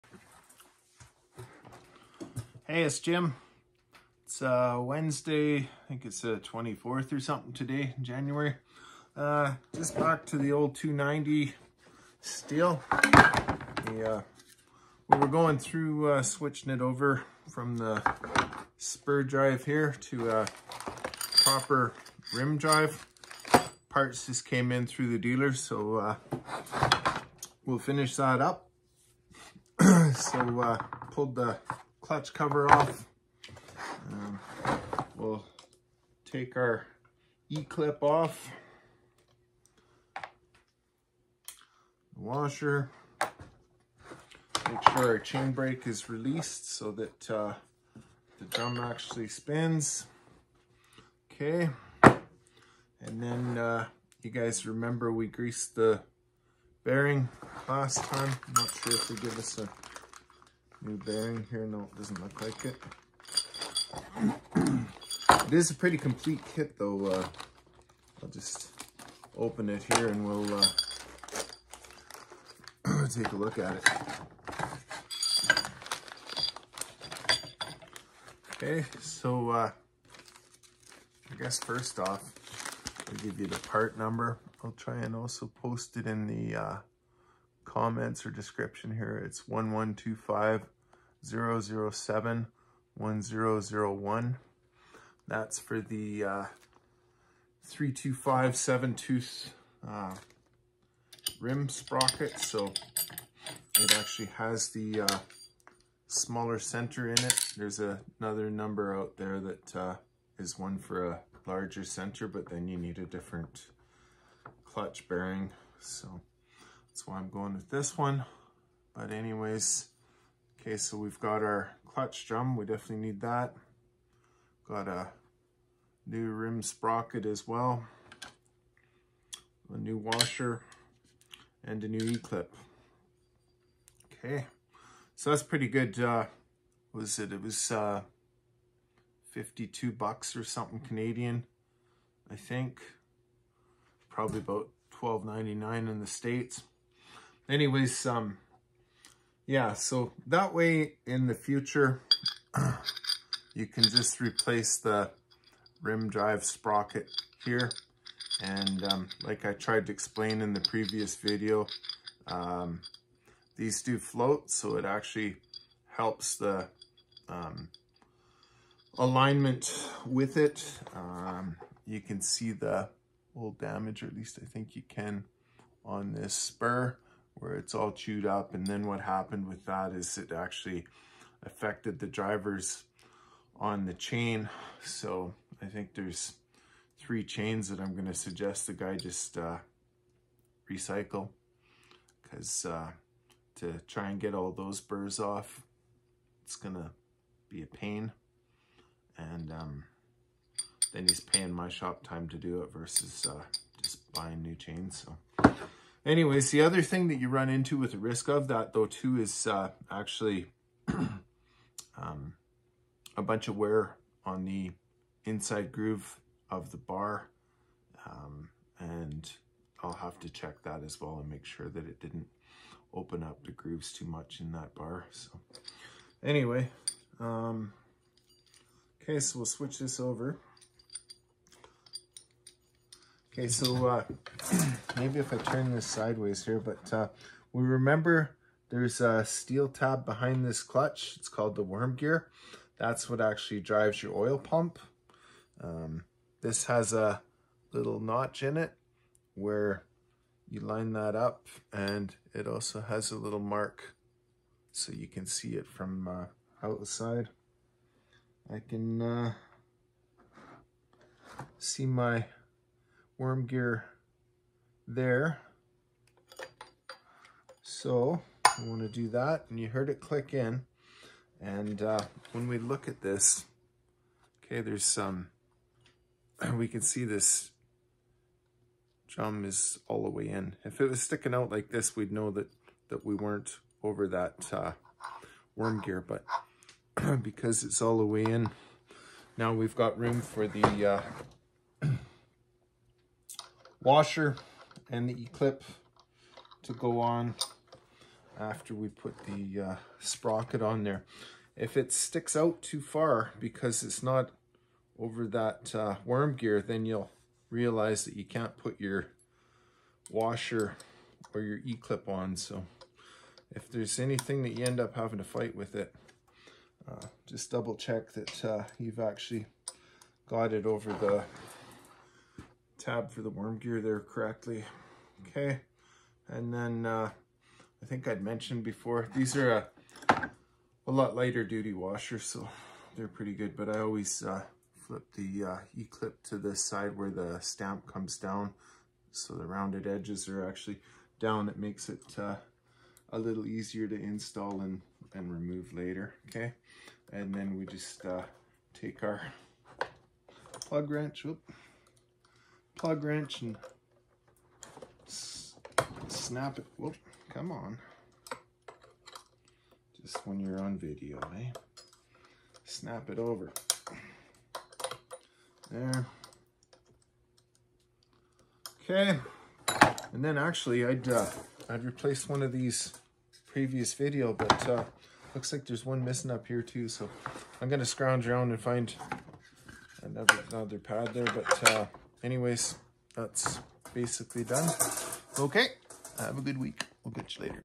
How to convert a Stihl MS 290 or 270/271 from spur to rim drive (clutch drum and sprocket)
Part numbers from Stihl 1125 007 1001 (includes clutch drum, 7 tooth small spline rim sprocket in 0.325, washer and e-clip)